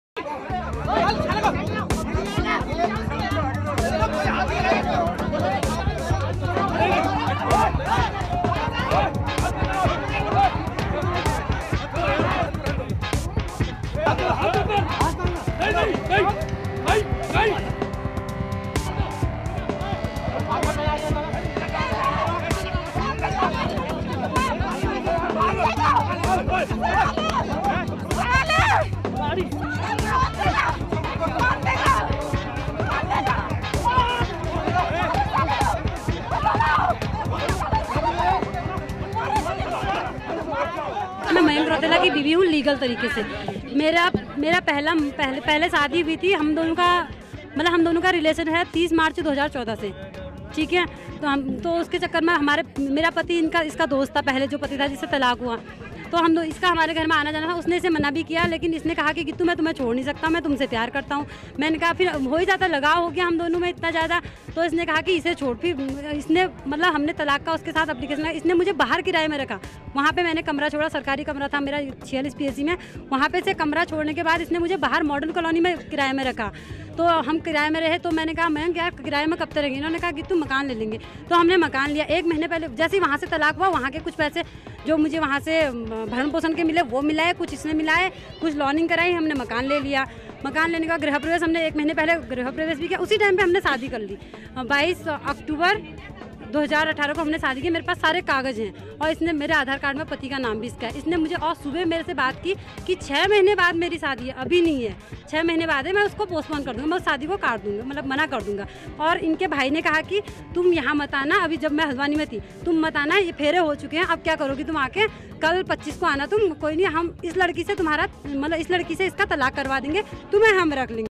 नैनीताल में दूसरी शादी करना पड़ा महंगा, पहली पत्नी ने की जमकर पिटाई | Navbharat News
नैनीताल में एक शख्स ने पहले तो तलाक कराकर शिकायतकर्ता महिला से शादी की.. बाद में उसे झूठ बोलकर खुद दूसरी शादी करने लगा। जैसे ही महिला को भनक पड़ी वो आनन फानन में रुद्रपुर से सीधा नैनीताल के होटल पहुंच गई और देखते ही देखते बवाल मच गया..

elcome to NAVBHARAT.